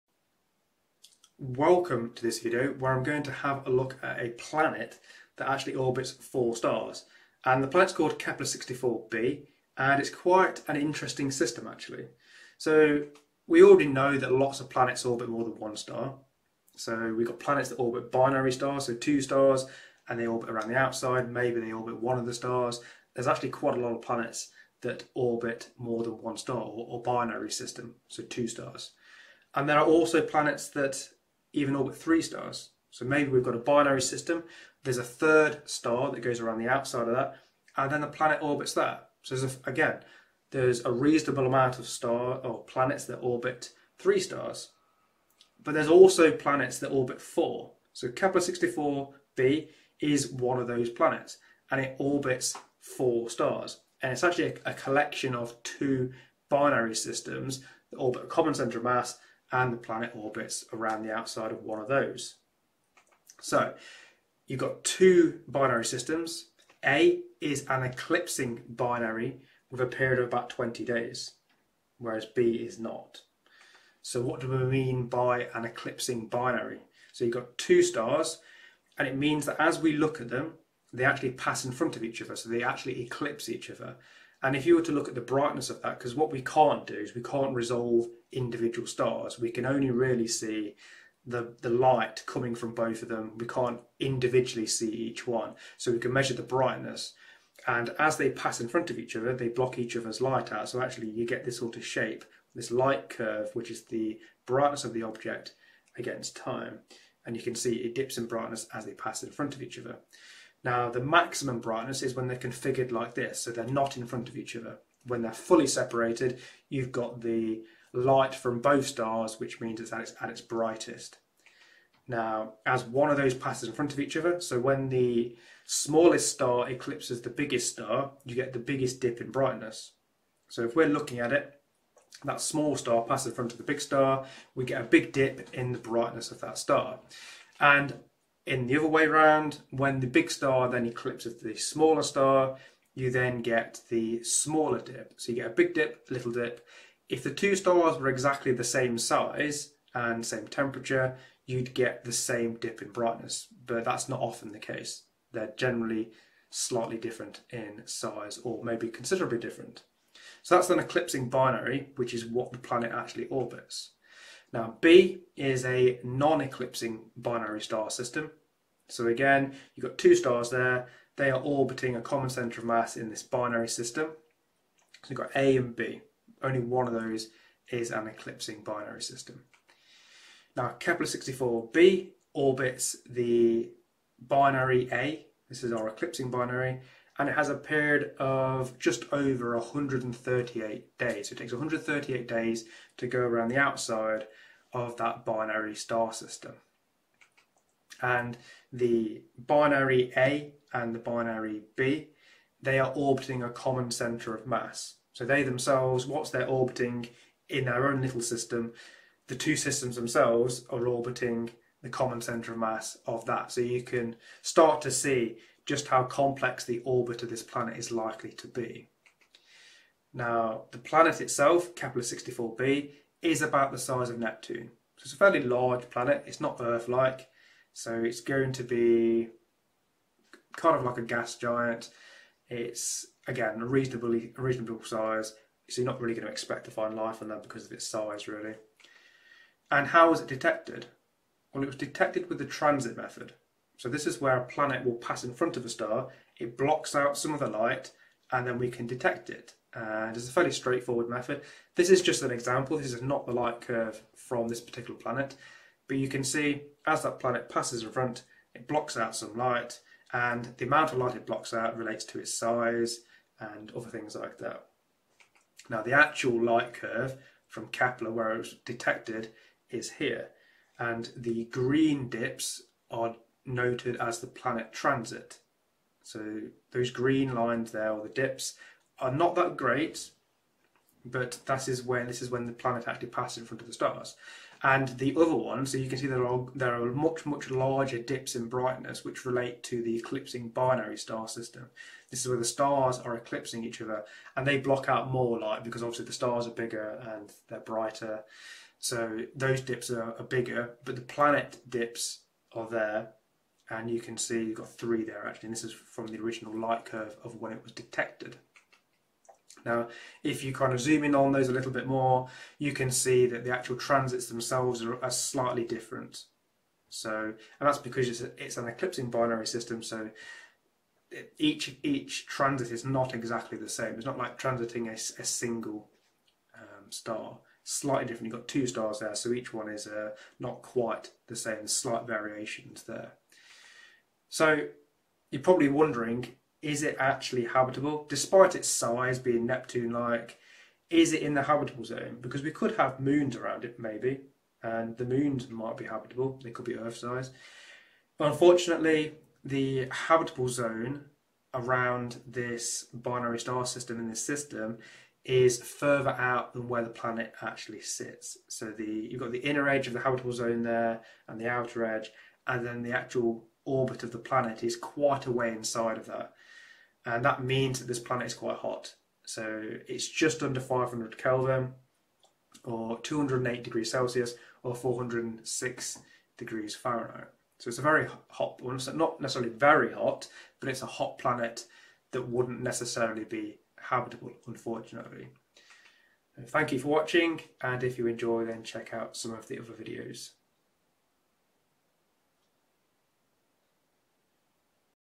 Kepler-64b: A Planet Orbiting 4 Stars
Welcome, my name is Phil, and in this video I look at the exoplanet Kepler-64b which orbits 4 stars.

Many exoplanets have now been discovered around more than star. While most of the orbit 2 stars, with a handful orbiting 3 stars, there are planets that orbit 4 stars. Kepler-64b is one of those planets and was detected by the Kepler Space Telescope as it transited its host stars.

The star system is comprised of a pair of binary star systems that orbit a common centre of mass. The binary pair A is an eclipsing binary star, where the stars pass in front of each other as viewed from Earth.